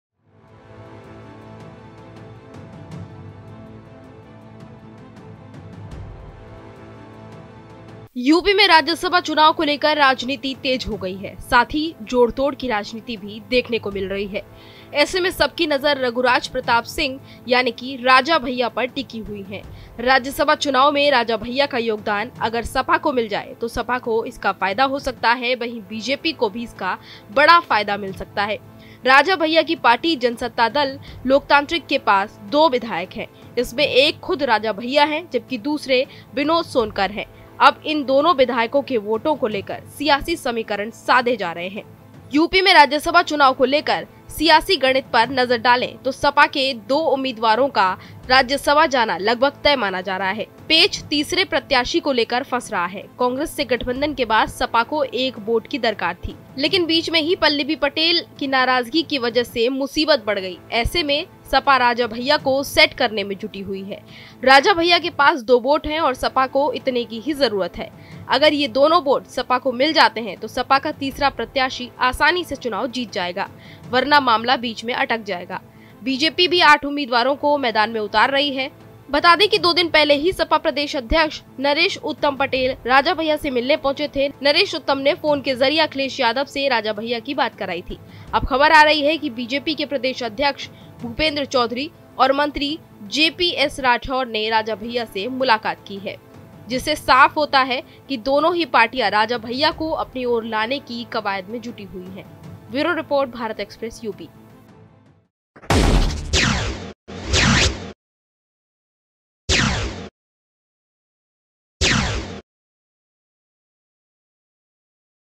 Raja Bhaiya को राजी करने में लगीं पार्टियां, जिस ओर जाएंगे जीत दिलाएंगे! |Rajyasabha Election
यूपी में राज्यसभा चुनाव को लेकर सियासत तेज हो गई है. साथ ही जोड़ तोड़ की राजनीति भी शुरु हो गई है। ऐसे में सबकी नजर रघुराज प्रताप सिंह उर्फ राजा भैया पर टिक हुई है। राज्यसभा चुनाव में राजा भैय्या का योगदान अगर सपा को मिल जाए तो सपा को इसका फायदा मिल सकता है।